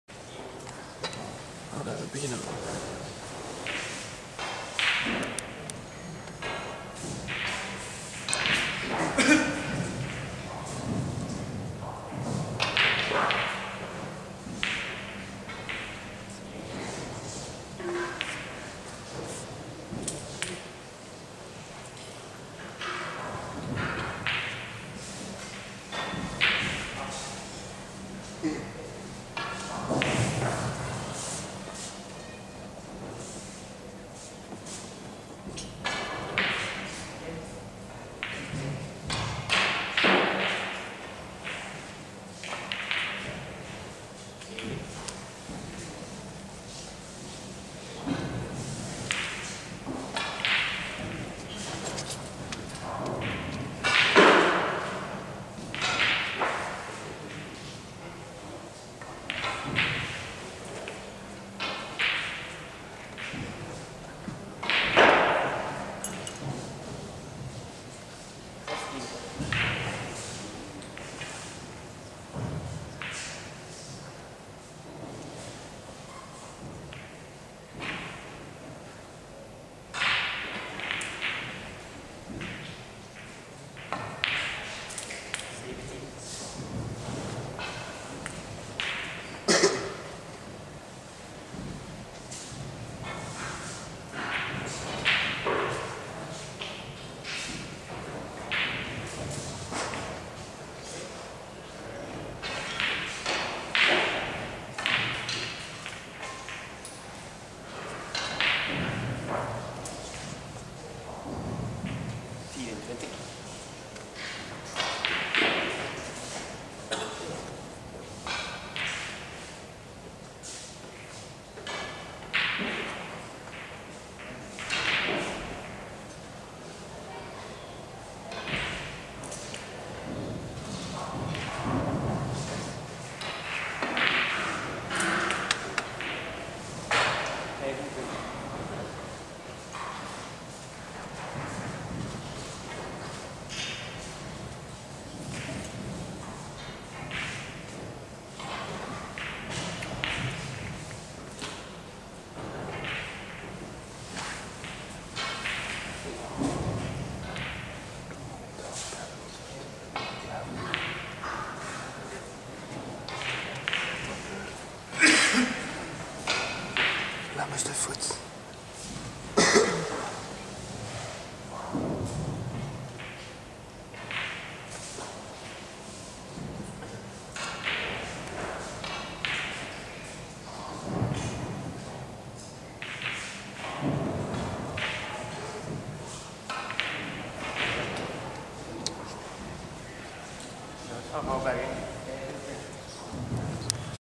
BK Dubbels - halve finale - Steve Lemmens
BK Dubbels 2010 - Sunparks Molse Meren
Steve Lemmens/Tim Van Goubergen vs Peter Bullen/Kevin Van Hove 4-5